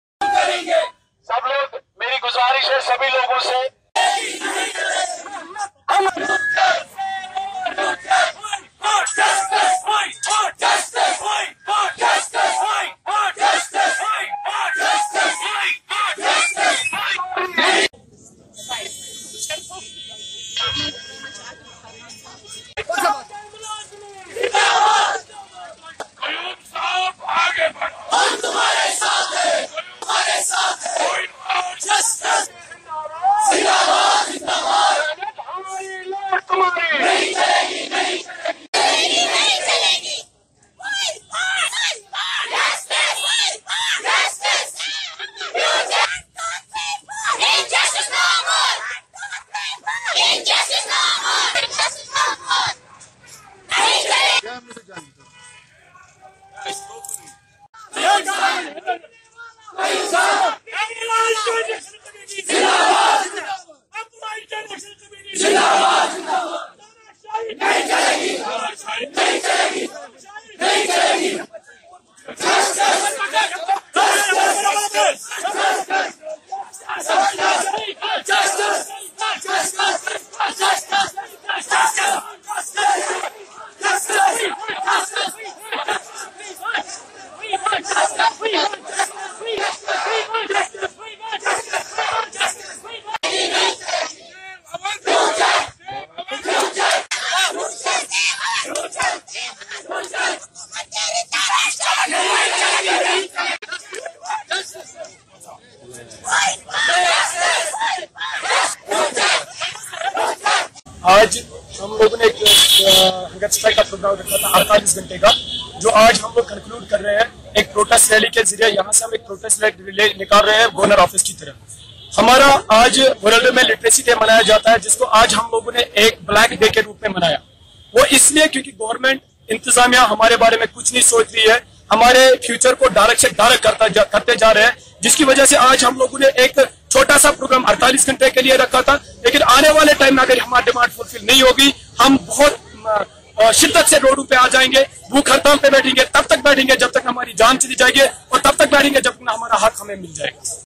SBM STAGED PROTEST RALLY FOILED BY POLICE. By Nasir Khan
SBM (serva shiksha abiyaan) STAGED PROTEST RALLY from Pratab Park to raj bhawab FOILED BY POLICE . they demanded regularization